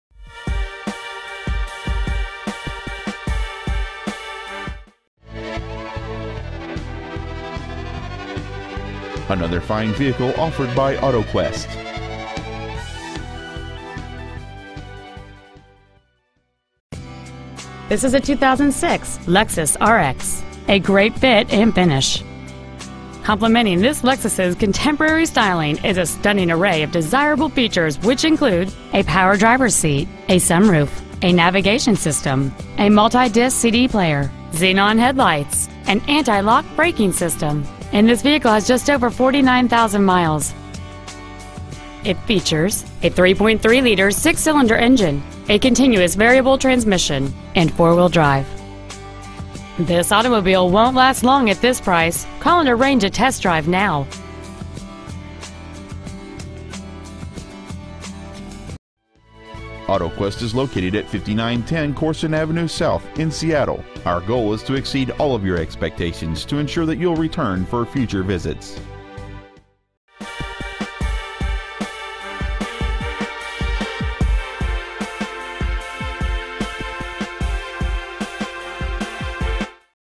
Used 2006 Lexus RX400h Seattle WA
We are proud to present this 2006 Lexus RX400h.

 Year: 2006
 Make: Lexus
 Model: RX400h
 Engine: 3.3L V6 Hybrid
 Trans.: CVT
 Exterior: Silver
 Miles: 49,900
 Interior: Black

 5910 Corson Ave S.

 Preowned 2006 Lexus RX400h Seattle WA